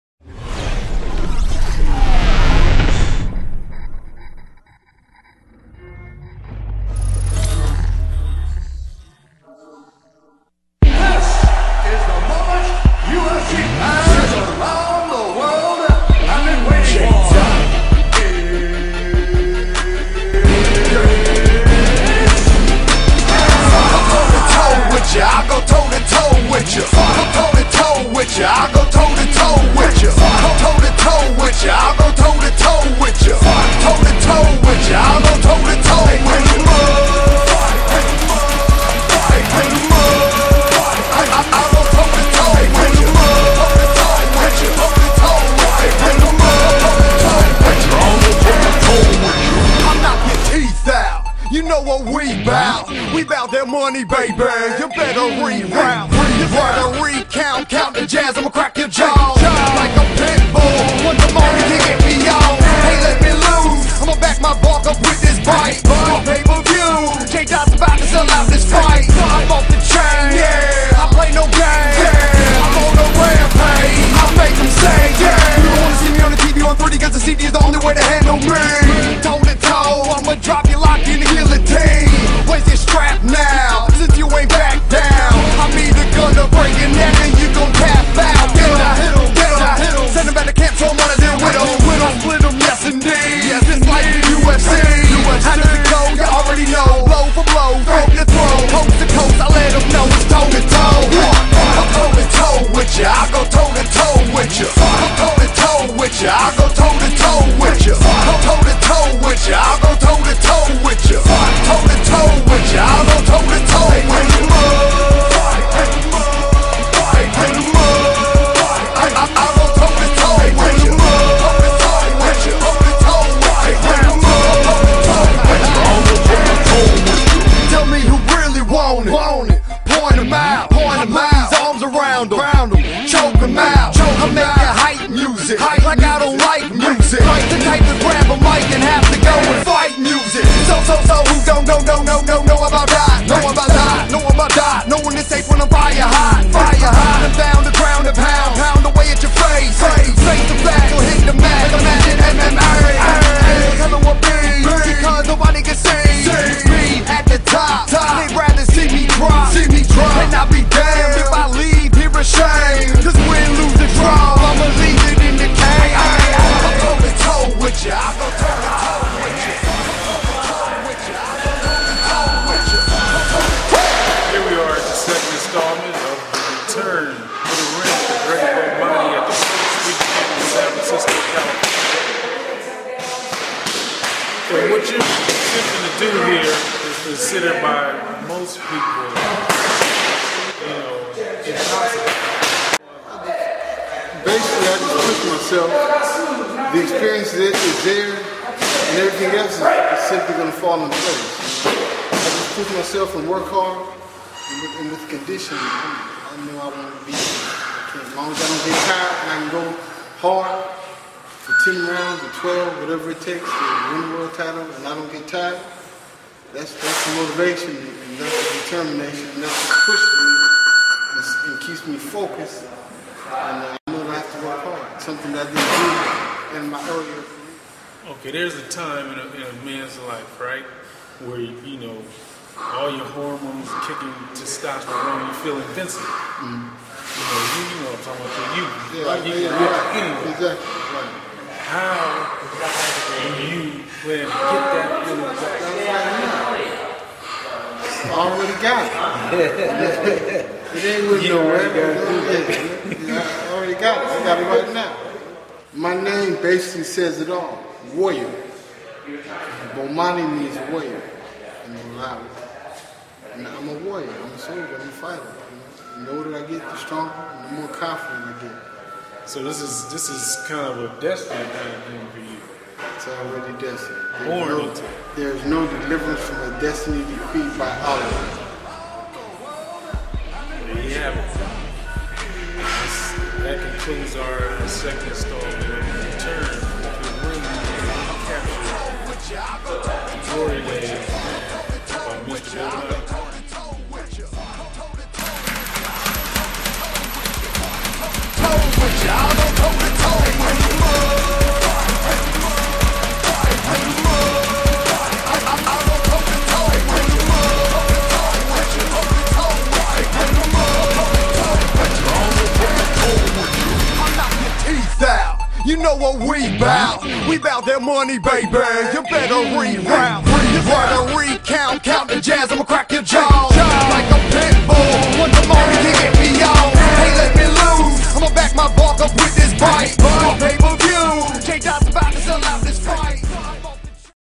Bomani Parker - Return to Greatness v2
The World Is About to witness the Comeback of Bomani Parker. An 88 Silver Medalist who toured Europe Extensively in the mid 80's. The only comeback that can rival this one is George Forman's. Ironically Bomani is roughly the same age as Forman was during his comeback.